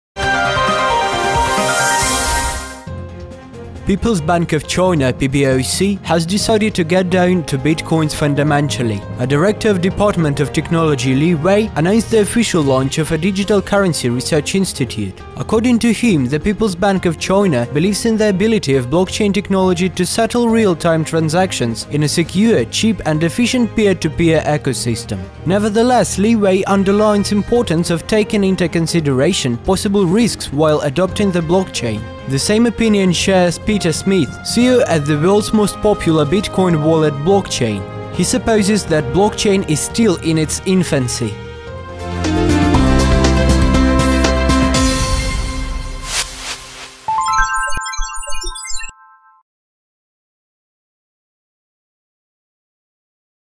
KCN Knowledge Institute of Chinese Bitcoins
People’s Bank of China (PBoC) has decided to get down to bitcoins fundamentally. A director of department of technology Li Wei announced the official launch of a digital currency research institute. According to him, the People’s Bank of China believes in the ability of blockchain technology to settle real-time transactions in a secure, cheap and efficient peer-to-peer ecosystem. Nevertheless Li Wei underlines importance of taking into consideration possible risks while adopting the blockchain. The same opinion shares Peter Smith, CEO at the wo